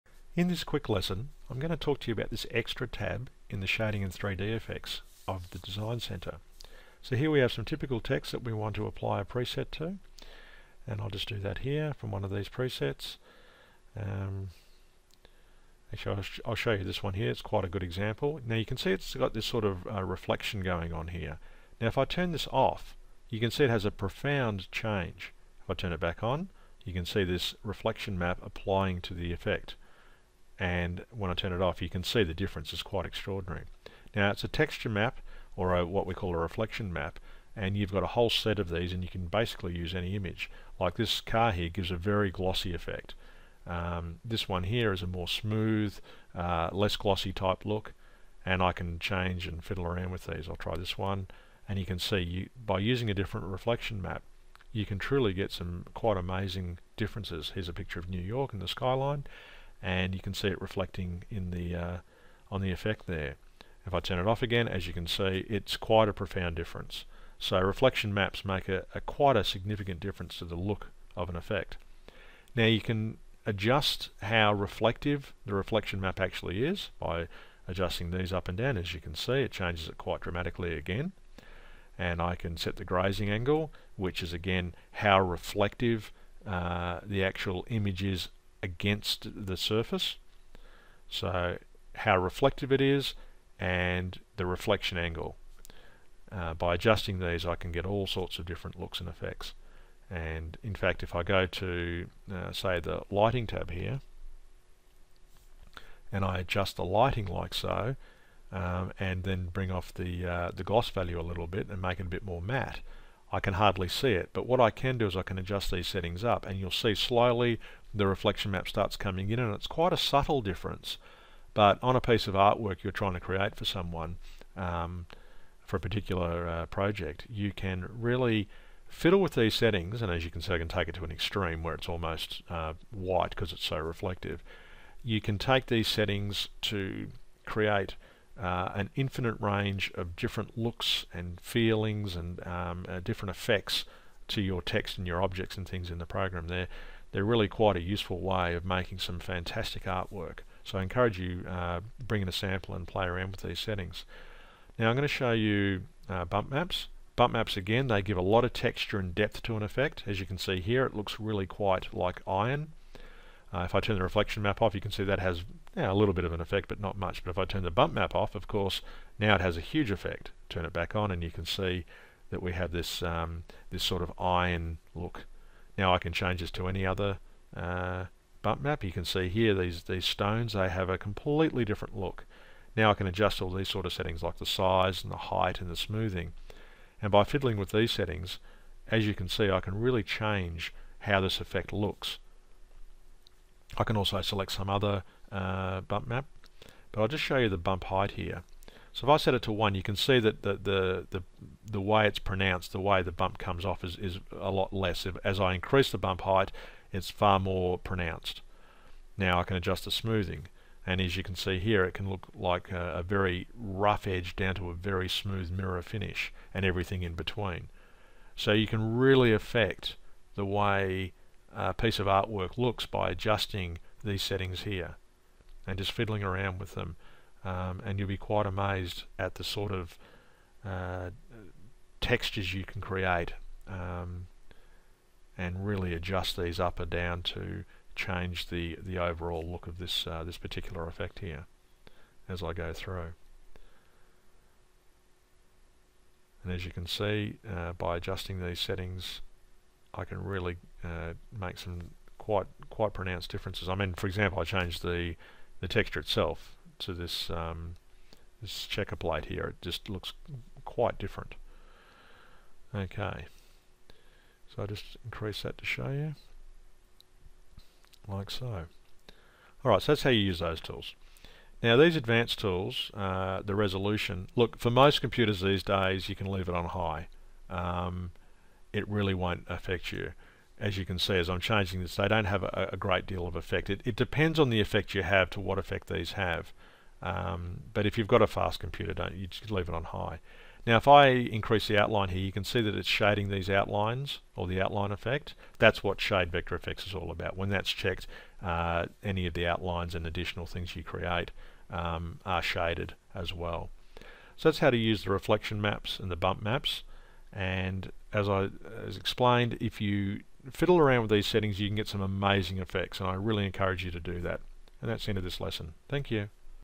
Quick Lesson: DesignCenter - Refelction & Bump Maps for Special and 3D Effects
A quick lesson on working with the Reflection and Bump Map tools from the Extras tab from the Shading and 3D Effects tool panel in the DesignCenter.